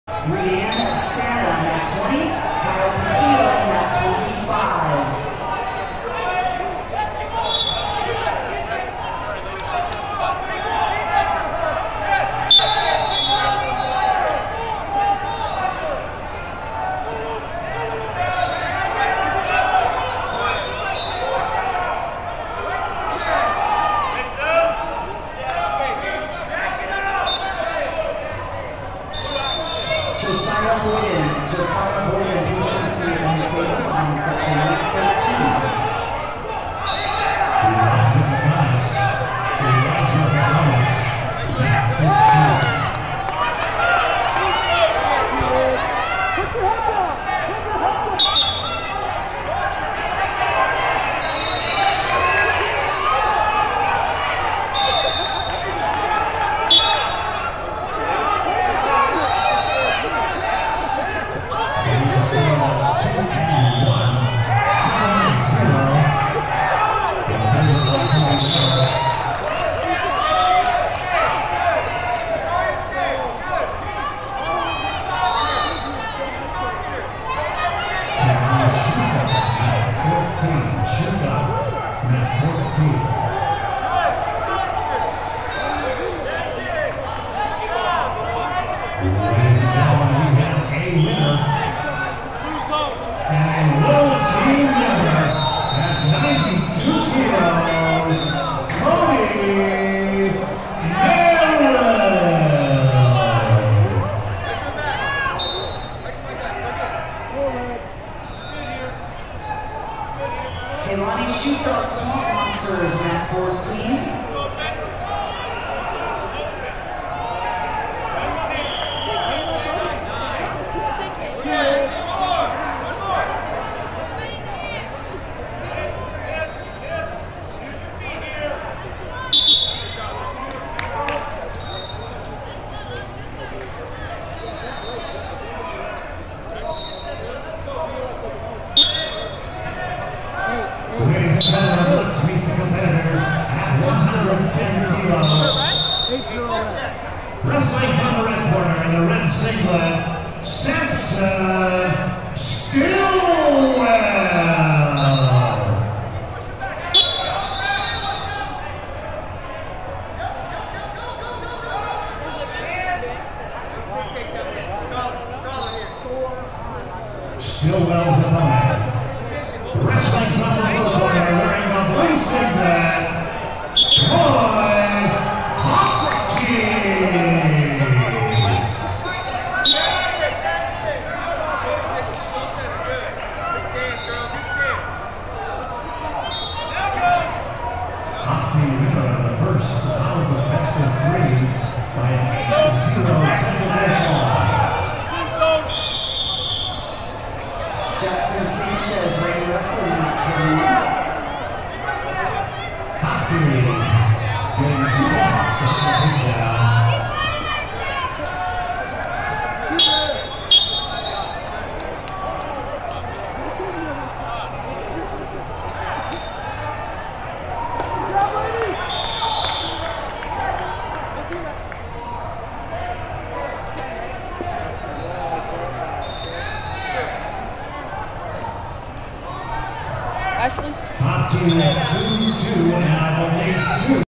112 Lbs Round Of 16 - Jenavi Alejandro, Aniciete Training Club Vs Zendell Oribello, Legends Of Gol
2022 USMC US Open Wrestling Championships - 112 Lbs Round Of 16 - Jenavi Alejandro, Aniciete Training Club Vs Zendell Oribello, Legends Of Gold Las Vegas E123